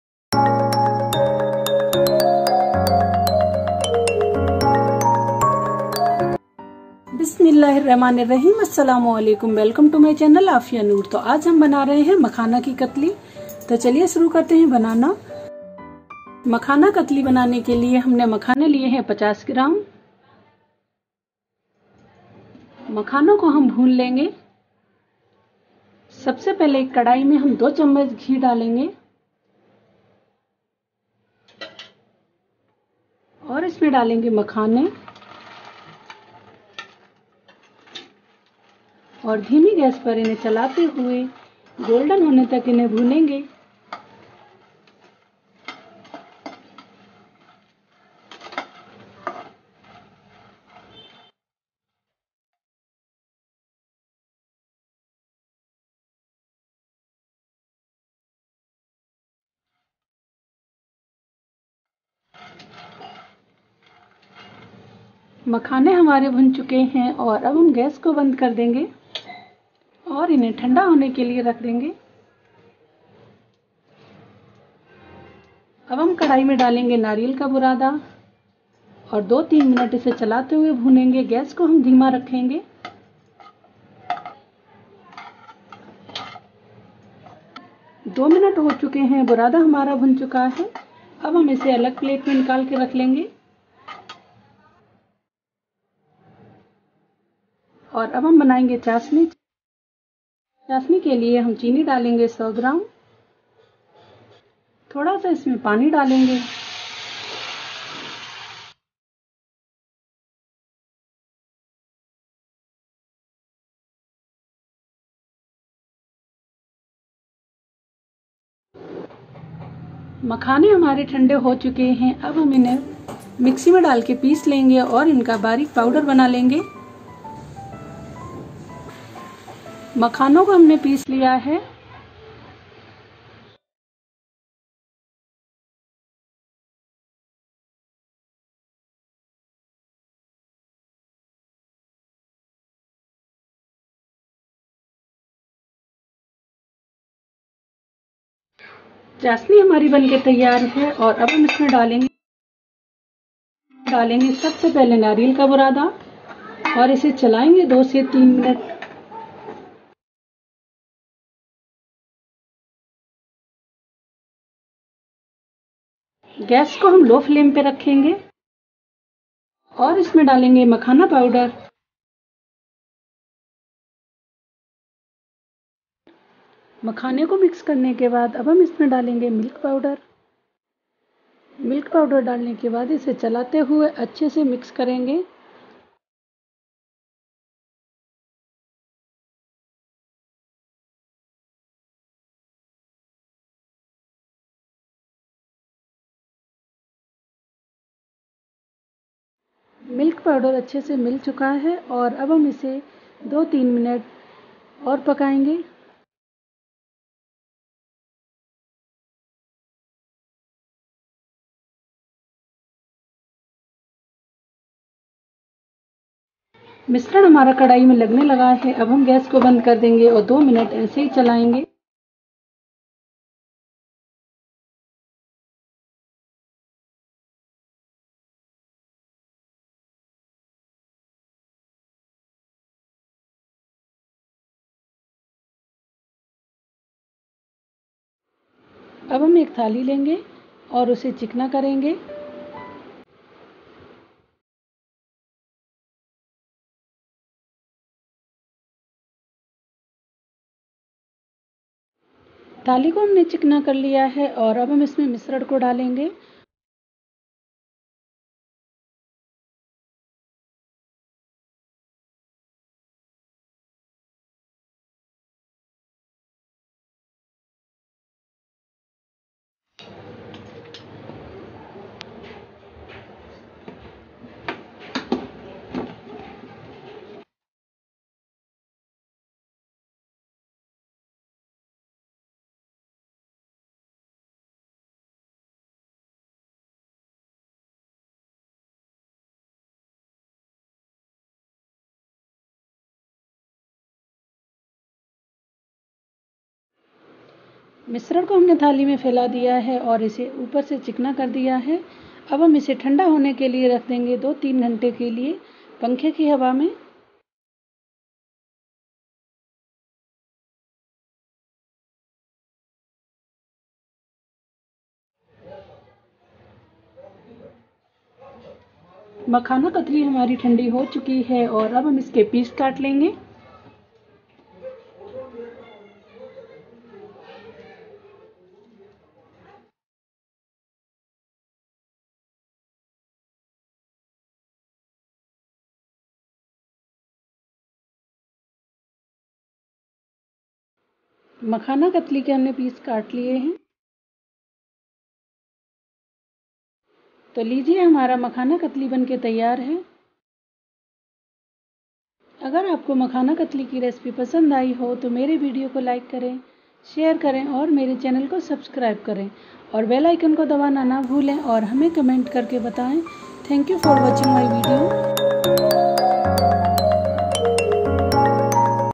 मखाना कतली रेशिपी
सामग्री-
 मखाने  50 ग्राम
घी   2 चम्मच
मिल्क पाउडर  ,50 ग्राम
चीनी ,100
   नारियल बुरादा 50 ग्राम

Method :
Mkhane 50g
ghee 2 T, soopn
Milk Powder 50g
Sugar  100g
Nariyal burada 50g

.SPREAD LOVE AND PEACE
             XOXO